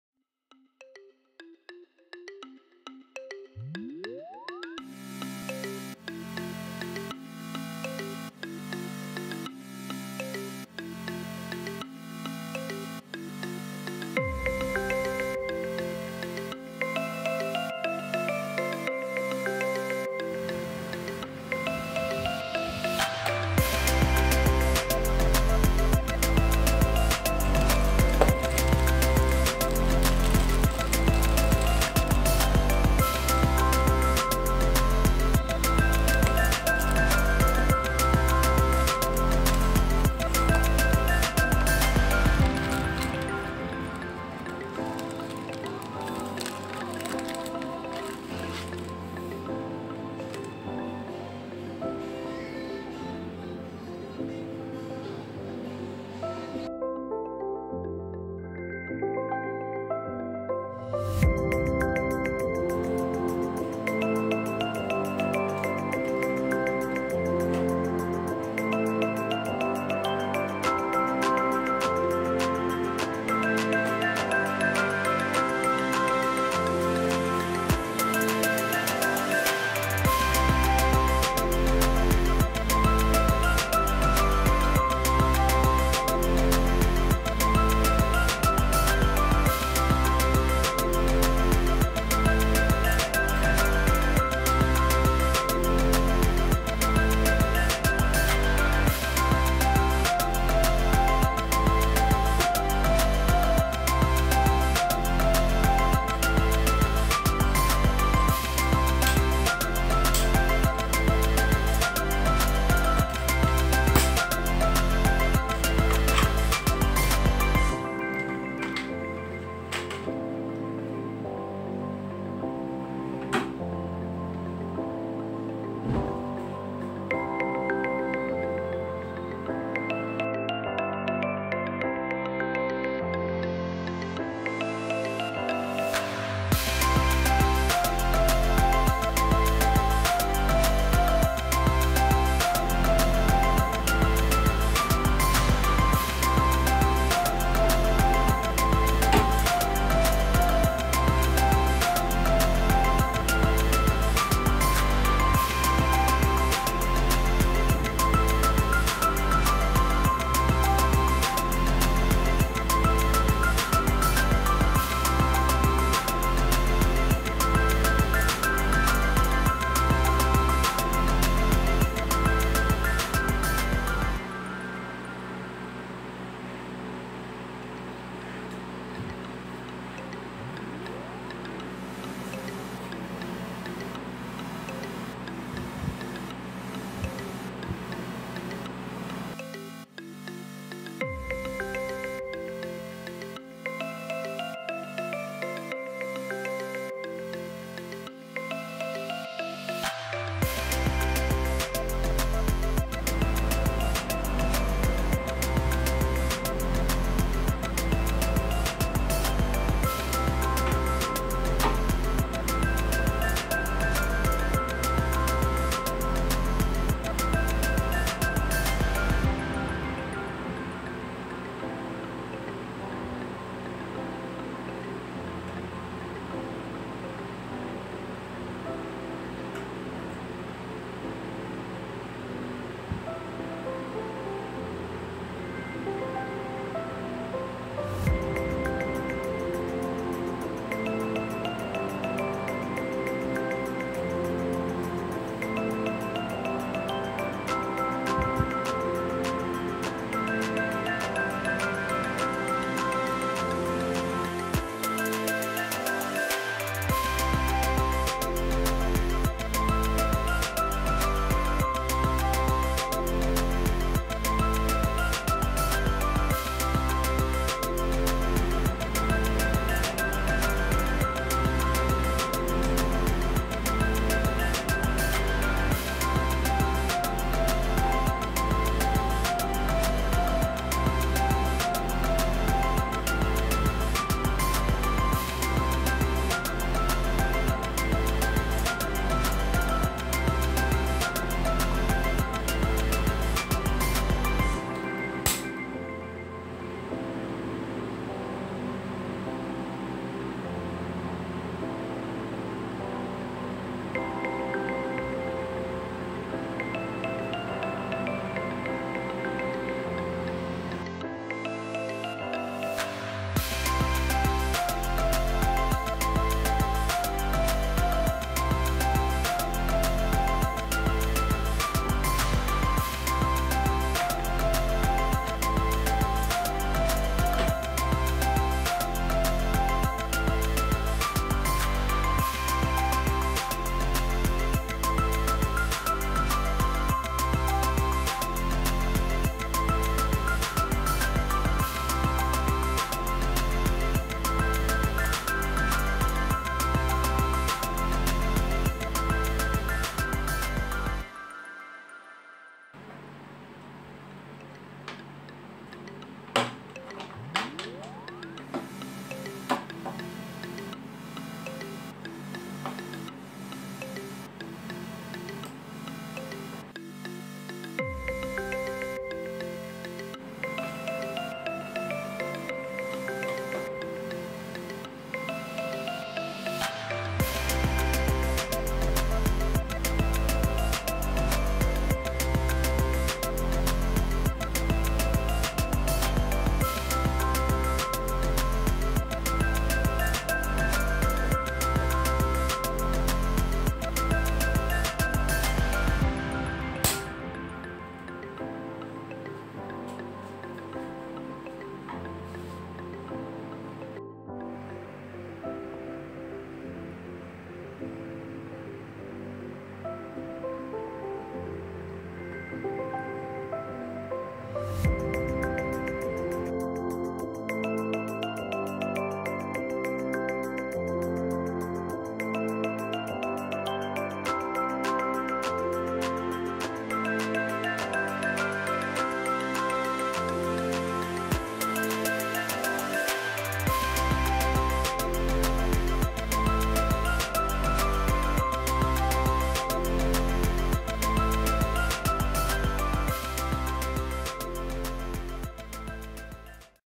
PZEM 015 battery capacity monitor installation (tamang pagkabit ng PZEM-015 monitor para sa baterya)
i have replaced my old voltmeter/ammeter with PZEM-015 battery capacity monitor. This meter provides you with battery capacity, voltage, current, power, energy, internal & external resistance reading as well as running time for the duration of the battery use.
This is a good way of monitoring your battery condition i real time.